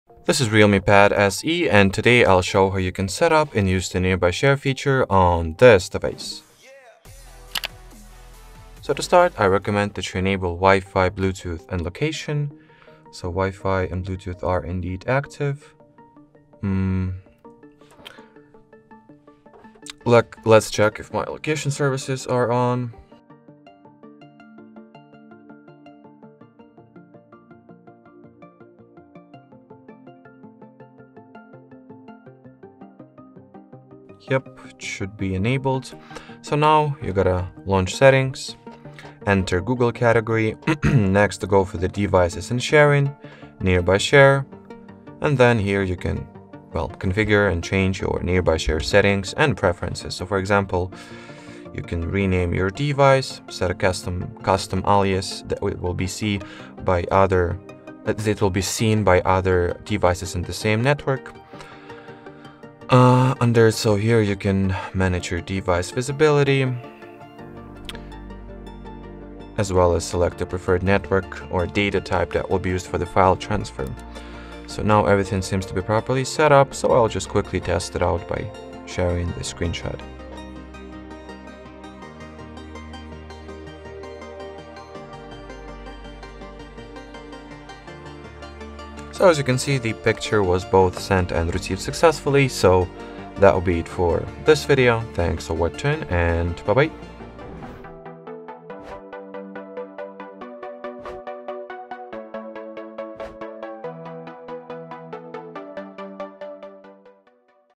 How to Set Up the Nearby Share Feature on REDMI Pad SE
Enhance your sharing experience on REDMI Pad SE! This video walks you through configuring and utilizing the Nearby Share feature. Discover seamless file sharing with just a few taps. Watch now to unlock the full potential of Nearby Share on your REDMI Pad SE and streamline your device connectivity.

How to configure the Nearby Share option on REDMI Pad SE? How to use Nearby Share service on REDMI Pad SE? How to transfer files and data via the Nearby Share feature on REDMI Pad SE?